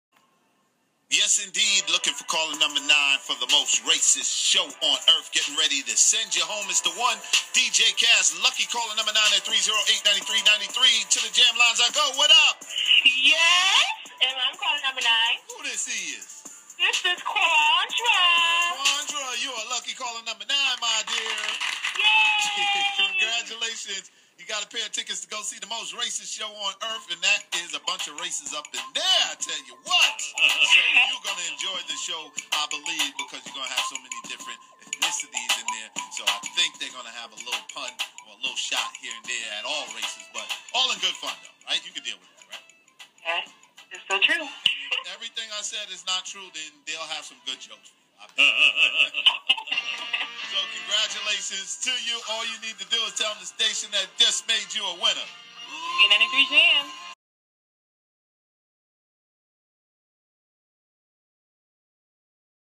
Z93 Jamz (Charleston, SC) MRSOE! Ticket Winner Dramatization
DJ Cass gives away tickets to MRSOE! on Z93 Jamz (Charleston, SC) and MRSOE! turned it into a Dramatization.  Enjoy!